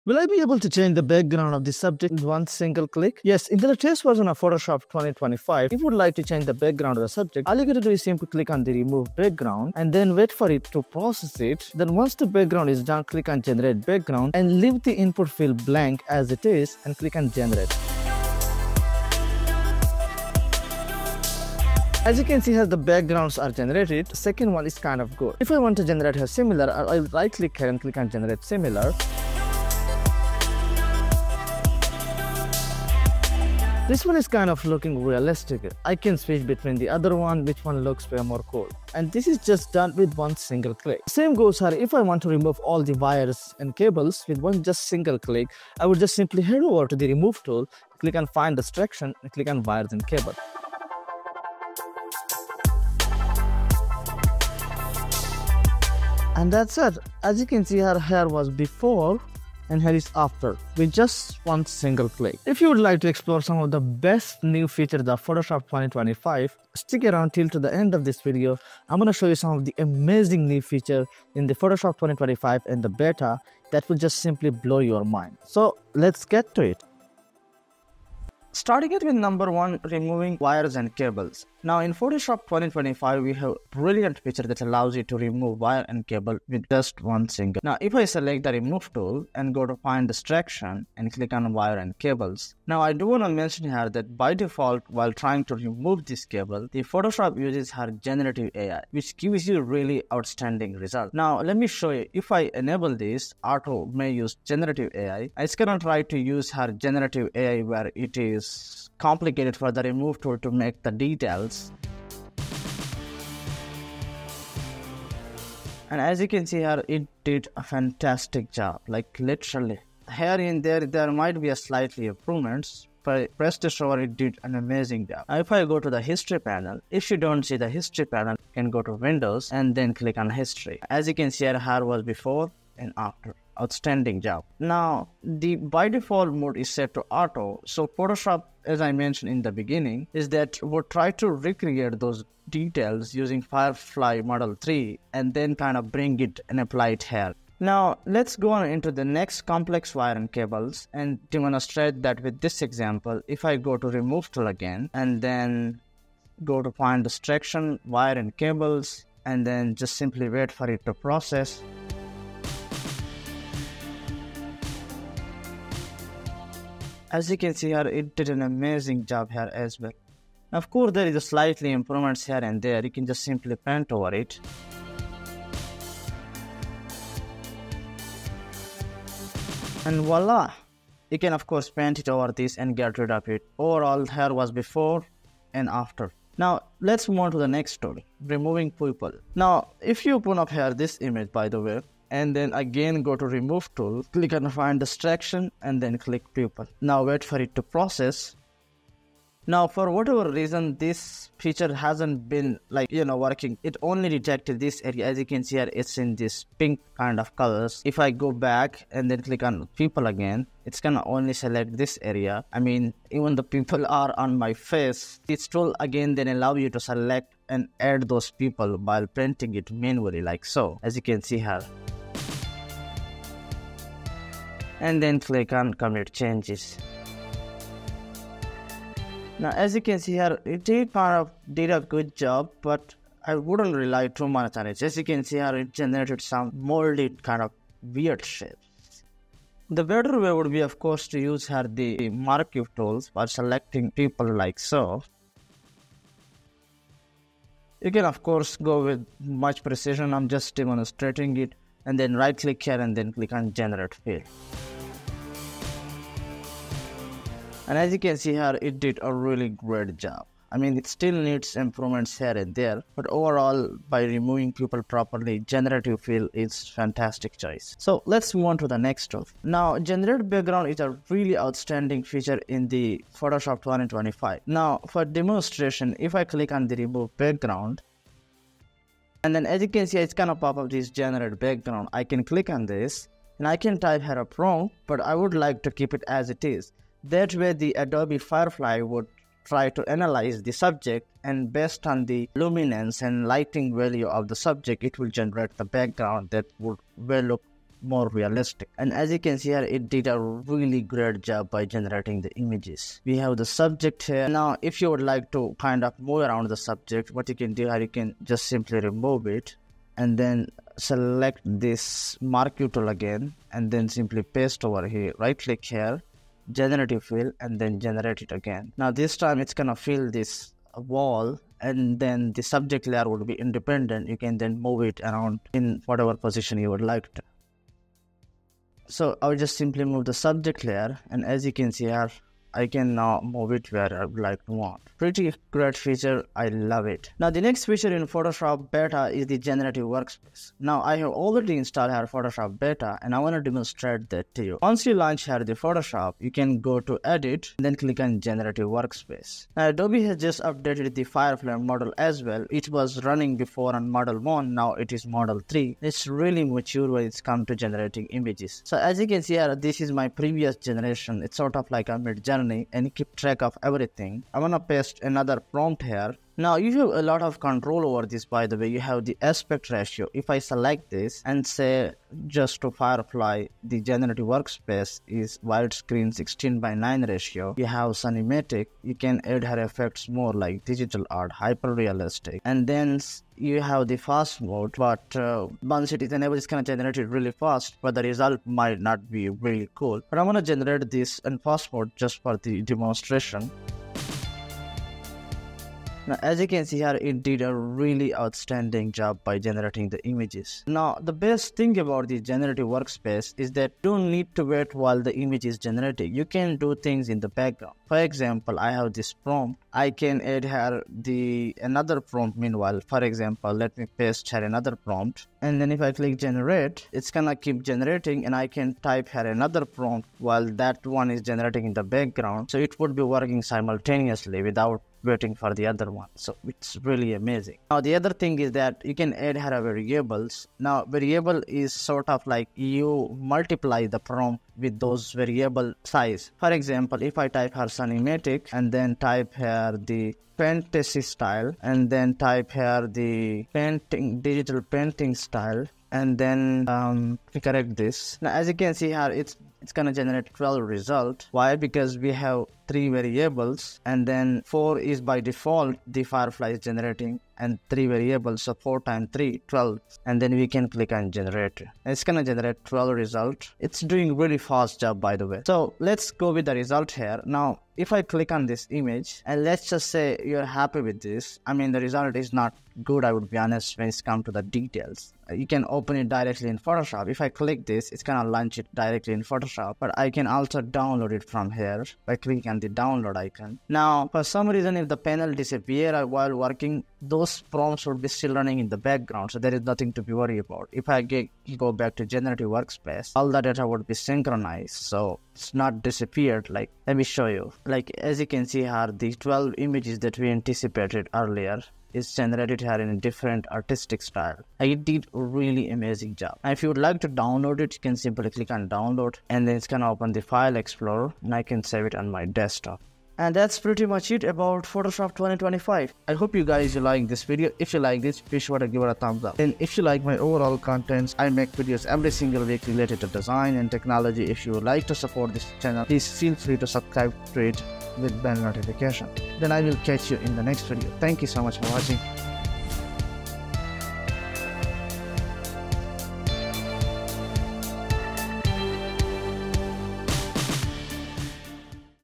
Photoshop 2025: Best 4 New Features and Updates Explained!
In this video, we’ll dive into the top new features in Adobe Photoshop 2025 that every designer needs to know! From the groundbreaking Generative AI tools to powerful new ways of removing unwanted objects, these updates are game-changers.

Time Stamps:
00:00 - Intro
01:18 - Removing Wires and Cable
03:10 - Removing People
04:29 - Generate Background
05:37 - Generative Workspace